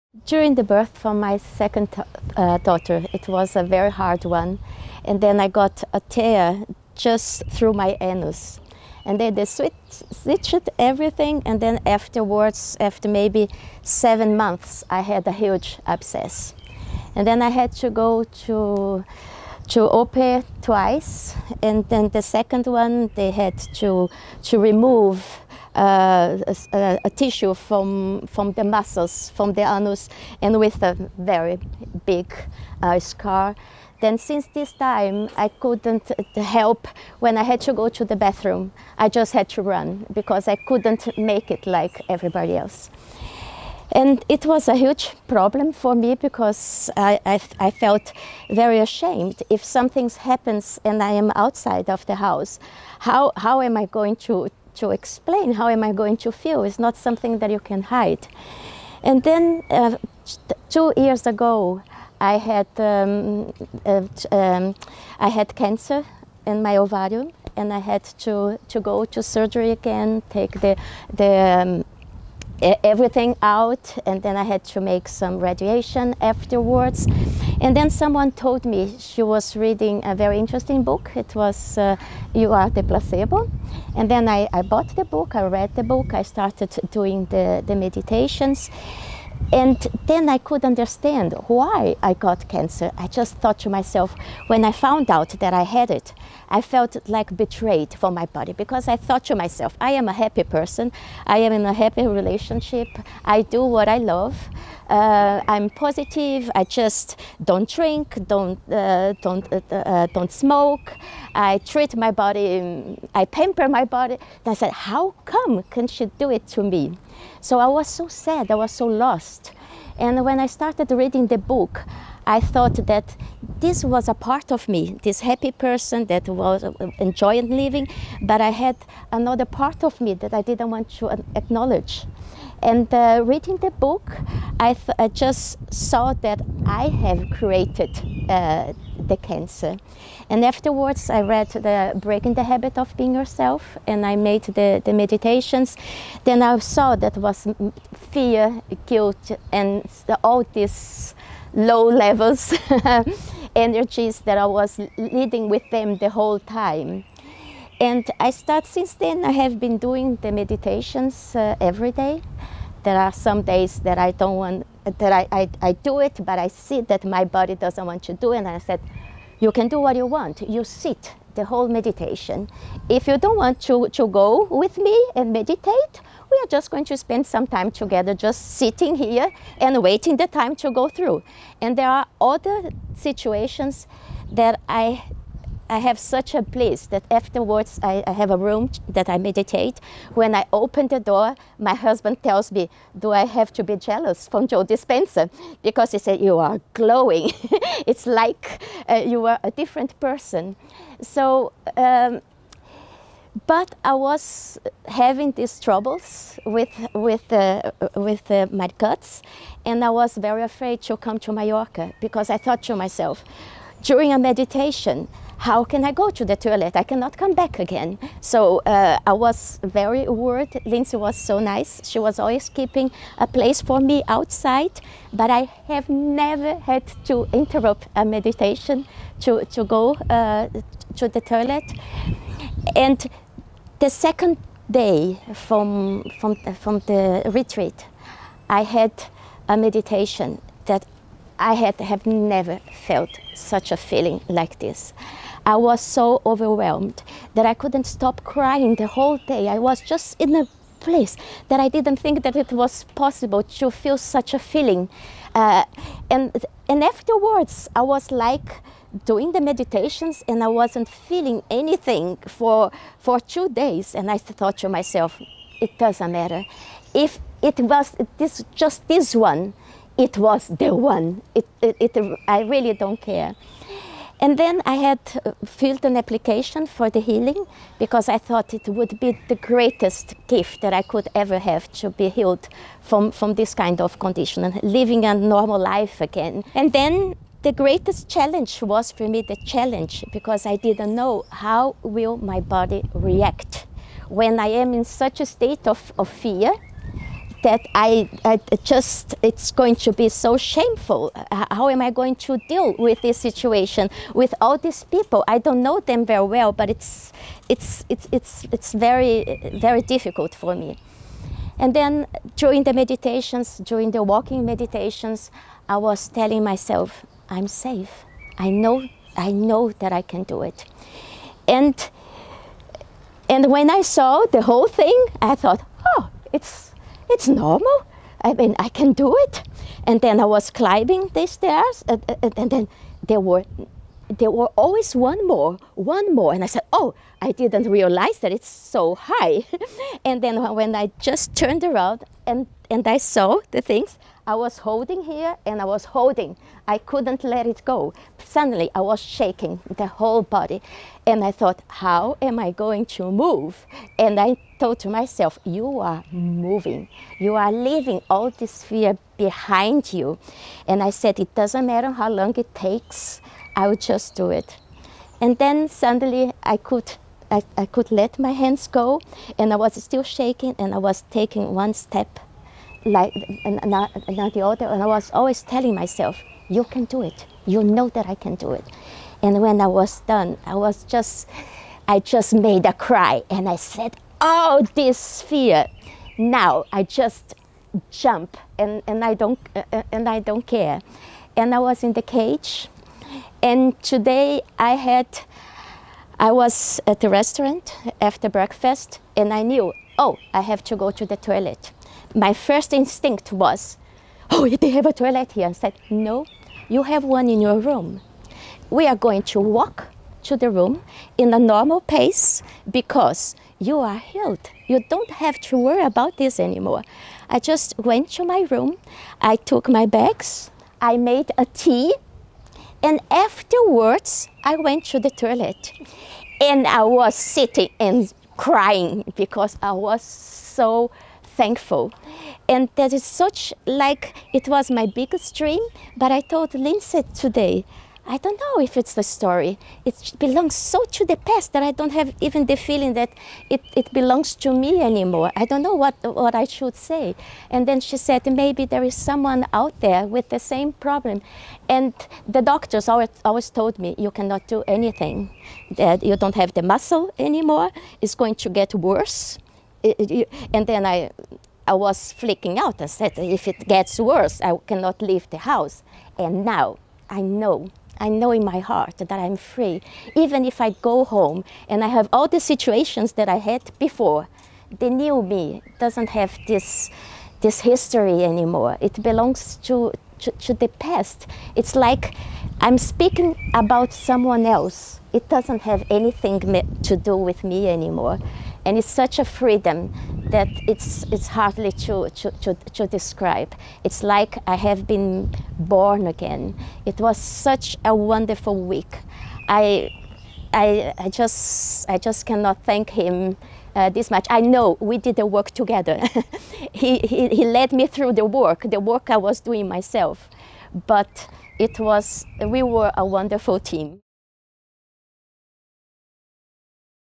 Stories of Transformation - Suelene
During childbirth, the tissues and muscle of Suelene's anus tore.  Doctors stitched the tear, but were not able to fix and reattach the muscle fibers, so that Suelene no longer had the ability to control her elimination functions.  Attending the Mallorca Advanced Week Long Retreat gave her many opportunities to confront her fears and overcome the limitations of her body.

Note - "The Cage" -- refers to the circle of 8 healers that surround a person to be healed during the Coherence Healing Mediations sessions in the advanced retreats.

Recorded at the Mallorca Advanced Week Long Retreat in April 2019 +